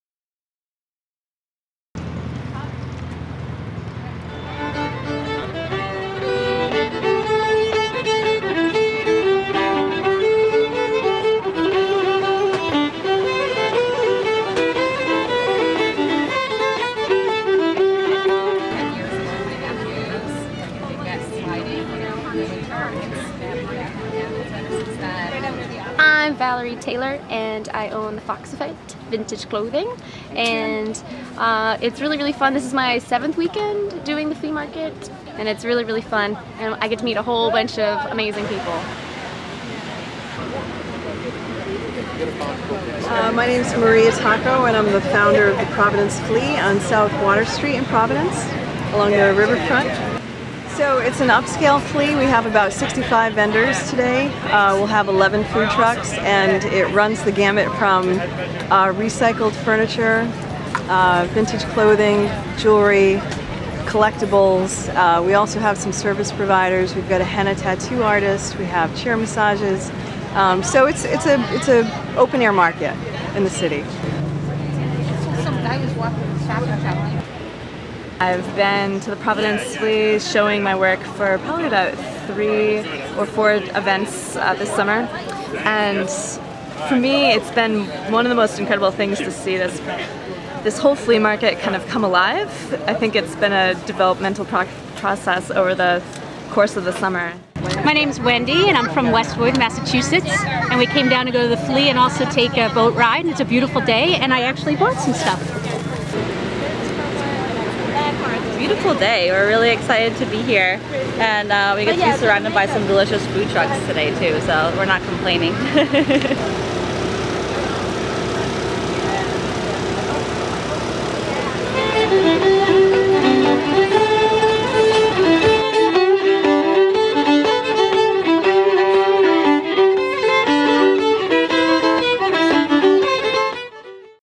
The Providence Flea shines
08.25.13: Hundreds turned out to support the Providence Flea, a marketplace filled with vintage clothes, furniture and collectables. Over 50 vendors set up each Sunday and the market will continue until mid-September.

Providence Journal video by Steve Szydlowski